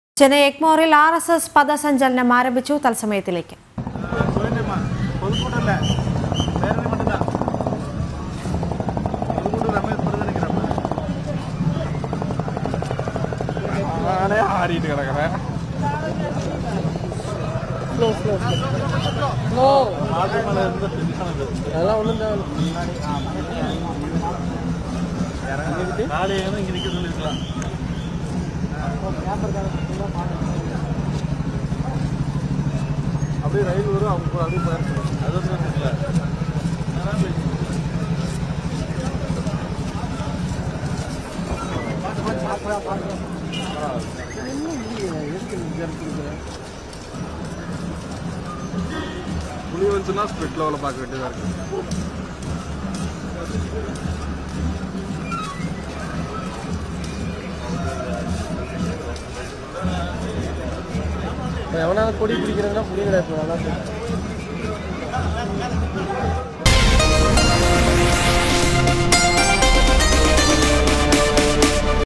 ചെന്നൈ എഗ്‍‍മോറിൽ RSS പദസഞ്ചലനം ആരംഭിച്ചു | CHENNAI | TAMILNADU
ചെന്നൈ എഗ്‍‍മോറിൽ ആർ എസ് എസ് പദസഞ്ചലനം ആരംഭിച്ചു.